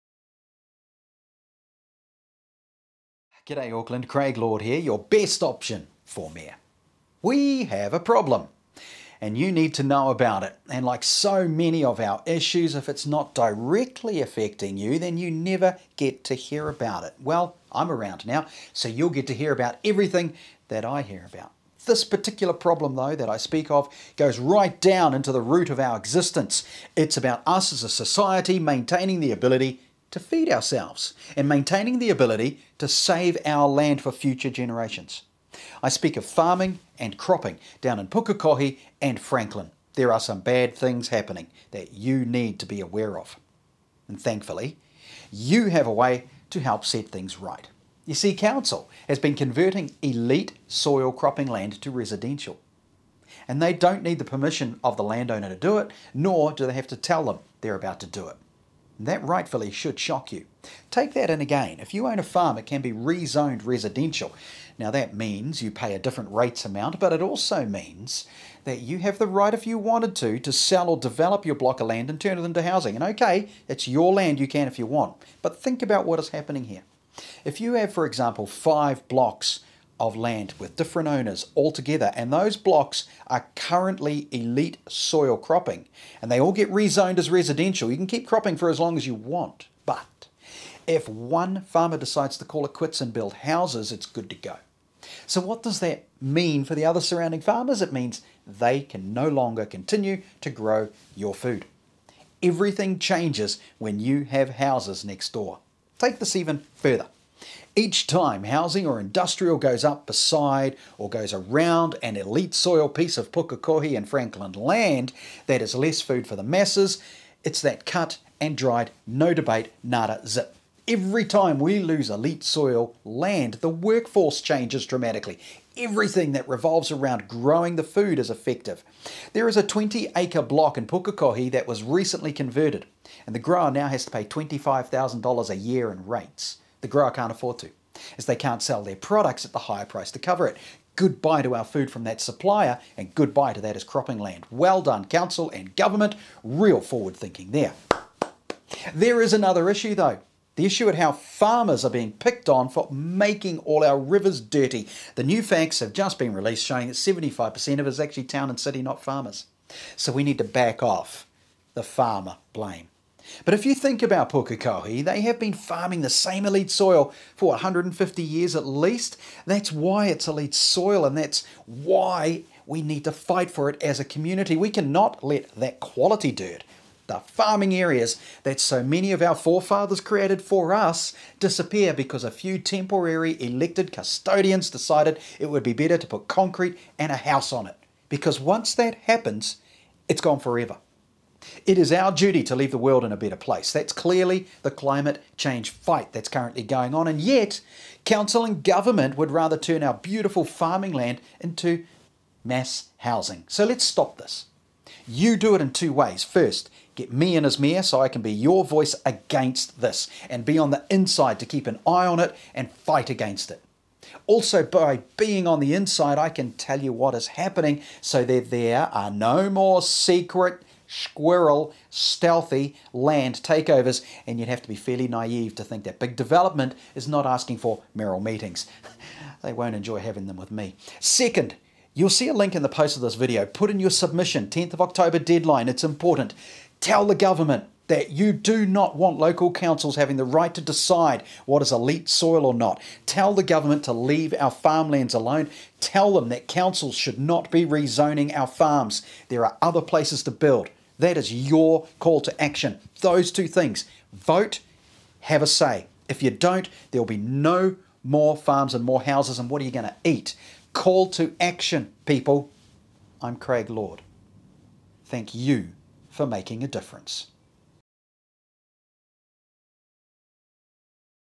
Wanna Eat Fresh Veges?
Craig Lord - Running for Auckland Mayor 2019.

You don't know what is going on because you are not told.
I am here to change that.
What is happening makes me angry - and you should be too.
We need to protect our land for now and for the future.
Temporary elected officials should not be doing what they are doing.
We are all custodians of this land and we should be doing the right thing.

Take a stand. You have a Call to Action!
Tick Craig Lord as the people's Mayor and together we can fight.

Don't waste the opportunity. Don't be apathetic.
VOTE so you can say you made a difference.